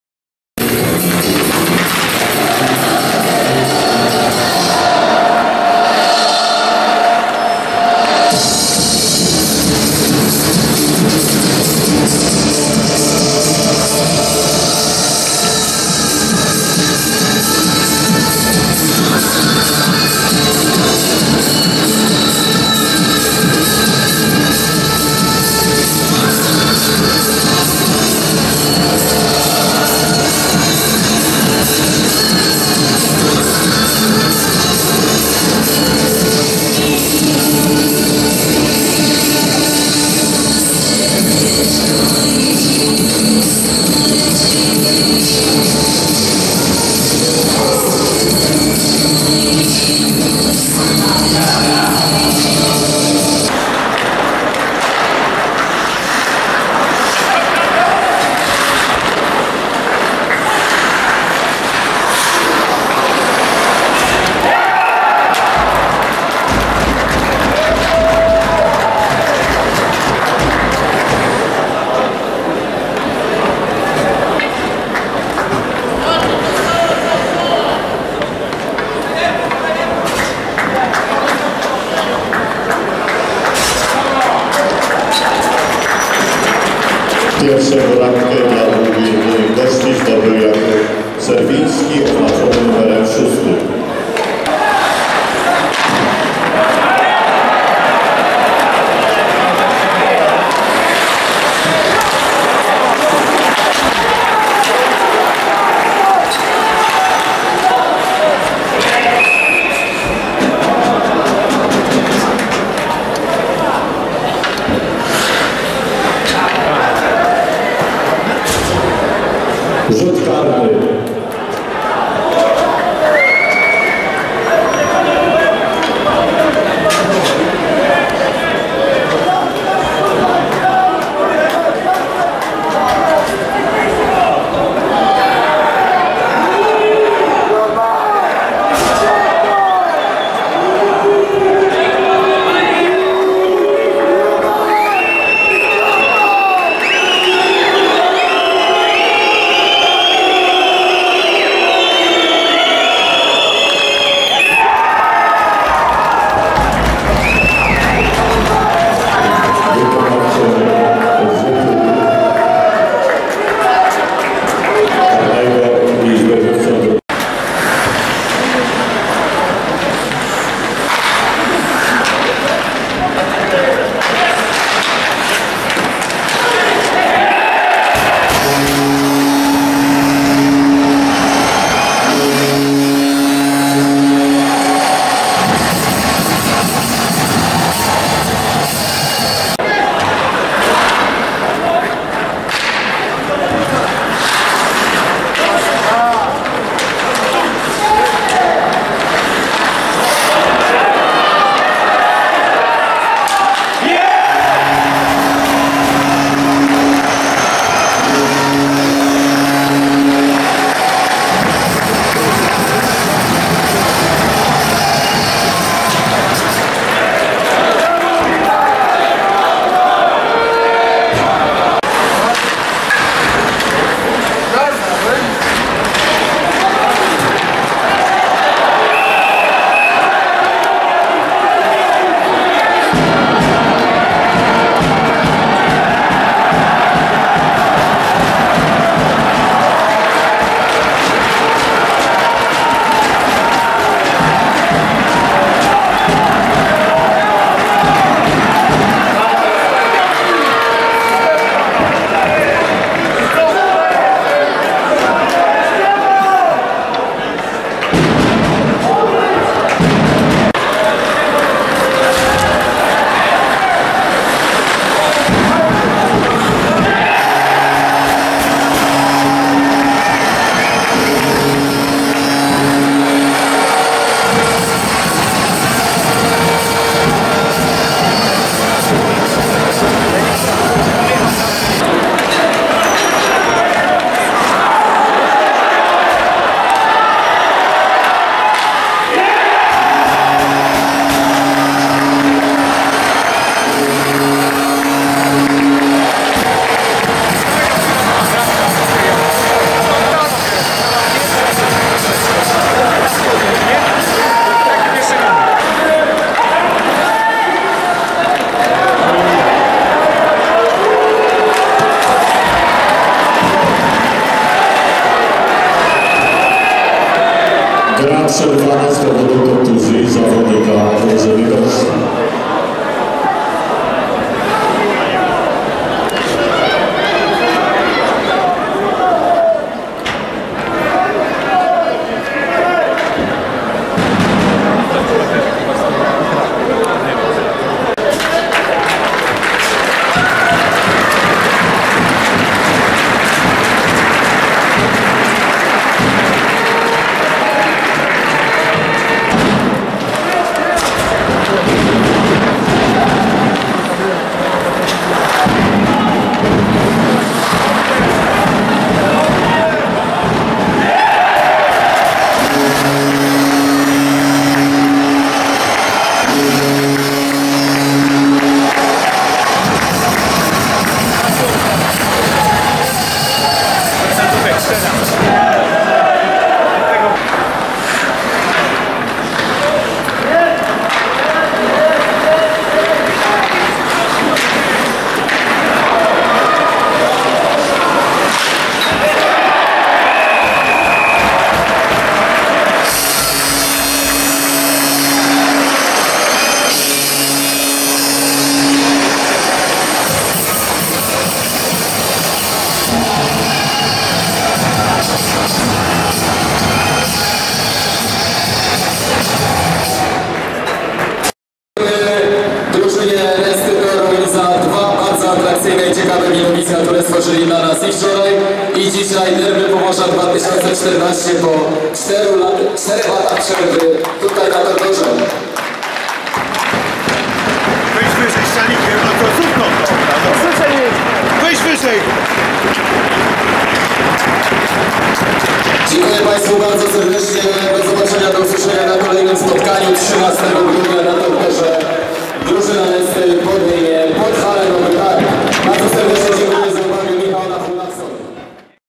16.11.2014 Nesta Toruń- MH Automatyka Stoczniowiec2014 [6:2]
Skrót meczu pierwszej ligi hokeja na lodzie pomiędzy zespołami TKH Nesta Toruń, a MH Automatyka Stoczniowiec2014 Gdańsk, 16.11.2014 Toruń.